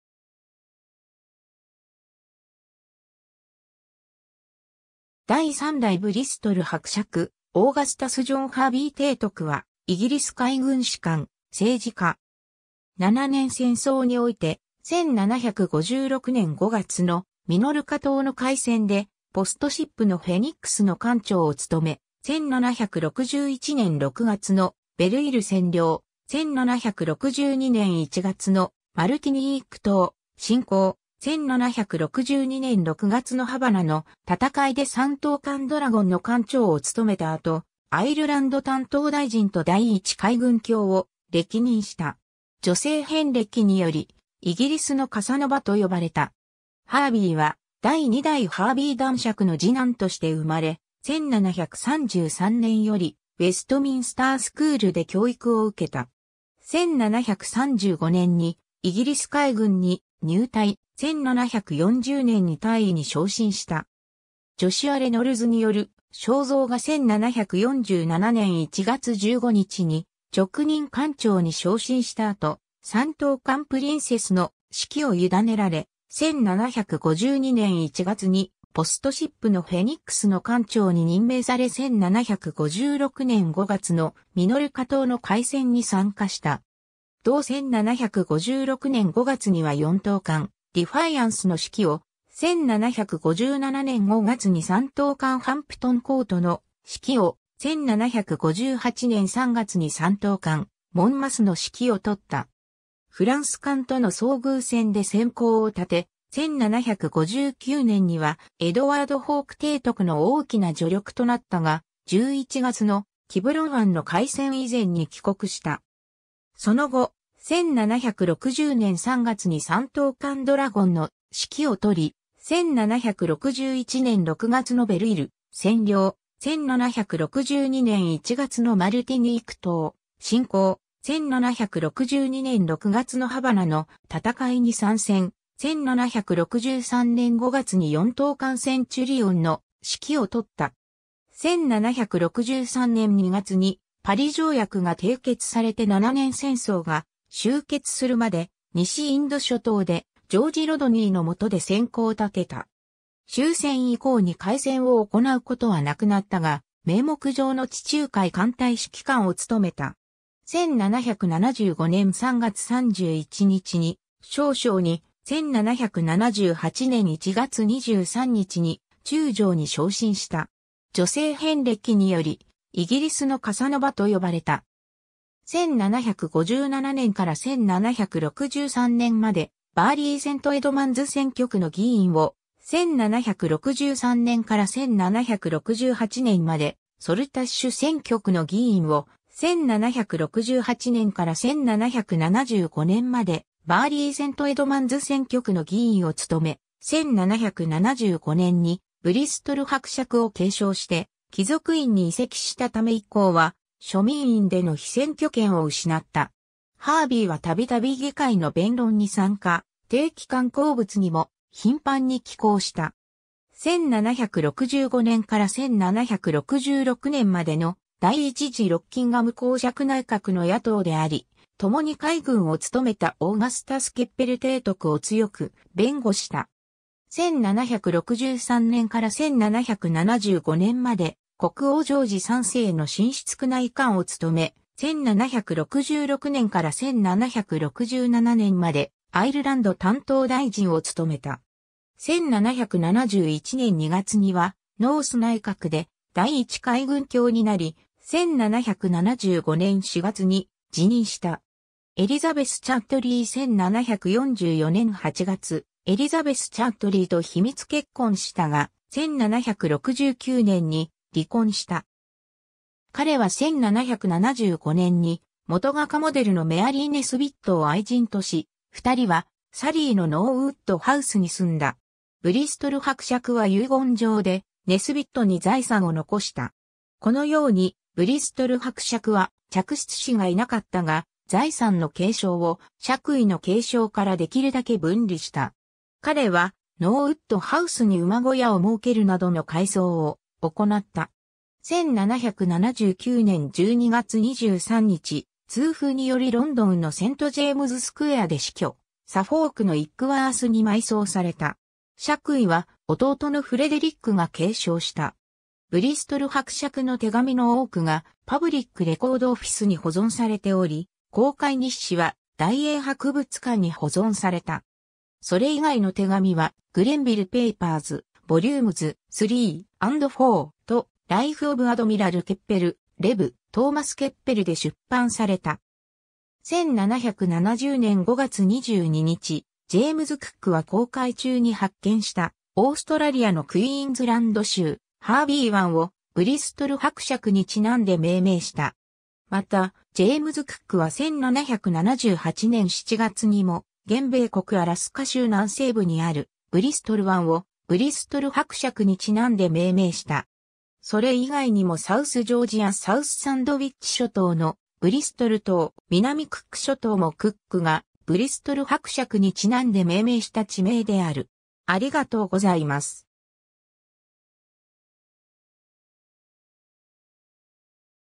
オーガスタス・ハーヴィー (第3代ブリストル伯爵)
第3代ブリストル伯爵オーガスタス・ジョン・ハーヴィー提督（英語: Augustus John Hervey, 3rd Earl of Bristol PC (Ire)、1724年5月19日 - 1779年12月23日）は、イギリス海軍士官、政治家。
七年戦争において、1756年5月のミノルカ島の海戦でポスト・シップのフェニックスの艦長を務め、1761年6月のベル＝イル占領、1762年1月のマルティニーク島侵攻、1762年6月のハバナの戦いで3等艦ドラゴンの艦長を務めた後、アイルランド担当大臣（在任：1766年 - 1767年）と第一海軍卿（在任：1771年 - 1775年）を歴任した。
女性遍歴により「イギリスのカサノヴァ」と呼ばれた。
 ハーヴィーは第2代ハーヴィー男爵の次男として生まれ、1733年よりウェストミンスター・スクールで教育を受けた。
1735年にイギリス海軍に入隊、1740年に大尉に昇進した。
 ジョシュア・レノルズによる肖像画 1747年1月15日に勅任艦長に昇進した後、3等艦プリンセスの指揮を委ねられ、1752年1月にポスト・シップのフェニックスの艦長に任命され1756年5月のミノルカ島の海戦に参加した。
同1756年5月には4等艦ディファイアンスの指揮を、1757年5月に3等艦ハンプトン・コートの指揮を、1758年3月に3等艦モンマスの指揮を執った。
 フランス艦との遭遇戦で戦功を立て、1759年にはエドワード・ホーク提督の大きな助力となったが、11月のキブロン湾の海戦以前に帰国した。
その後、1760年3月に3等艦ドラゴンの指揮を執り、1761年6月のベル＝イル占領、1762年1月のマルティニーク島侵攻、1762年6月のハバナの戦いに参戦、1763年5月に4等艦センチュリオンの指揮を執った。
 1763年2月にパリ条約が締結されて七年戦争が終結するまで、西インド諸島でジョージ・ロドニーの下で戦功を立てた。
終戦以降に海戦を行うことはなくなったが、名目上の地中海艦隊指揮官を務めた。
1775年3月31日に少将に、1778年1月23日に中将に昇進した。
女性遍歴により「イギリスのカサノヴァ」と呼ばれた（ハーヴィーとカサノヴァは同時代の人物である）。
 1757年から1763年までバーリー・セント・エドマンズ選挙区の議員を、1763年から1768年までソルタッシュ選挙区の議員を、1768年から1775年までバーリー・セント・エドマンズ選挙区の議員を務め、1775年にブリストル伯爵を継承して貴族院に移籍したため以降は庶民院での被選挙権を失った。
ハーヴィーはたびたび議会の弁論に参加、定期刊行物にも頻繁に寄稿した。
1765年から1766年までの第1次ロッキンガム侯爵内閣の野党であり、ともに海軍を務めたオーガスタス・ケッペル提督を強く弁護した。
1763年から1775年まで国王ジョージ3世の寝室宮内官を務め、1766年から1767年までアイルランド担当大臣を務めた。
1771年2月にはノース内閣で第一海軍卿になり、1775年4月に辞任した。
 エリザベス・チャッドリー 1744年8月、エリザベス・チャッドリー（1720年 - 1788年、後のキングストン＝アポン＝ハル公爵夫人）と秘密結婚したが、1769年に離婚した。
彼は1775年に元画家モデルのメアリー・ネスビットを愛人とし、2人はサリーのノーウッド・ハウスに住んだ。
ブリストル伯爵は遺言状でネスビットに財産を残した。
このように、ブリストル伯爵は嫡出子がいなかったが、財産の継承を爵位の継承からできるだけ分離した。
彼はノーウッド・ハウスに馬小屋を設けるなどの改装を行った。
1779年12月23日、痛風によりロンドンのセント・ジェームズ・スクエアで死去、サフォークのイックワースに埋葬された。
爵位は弟のフレデリックが継承した。
 ブリストル伯爵の手紙の多くがパブリック・レコード・オフィス（現イギリス国立公文書館の前身）に保存されており、航海日誌は大英博物館に保存された。
それ以外の手紙はGrenville Papers, vols. iii. and iv. (London, 1852–1853)とLife of Admiral Kep...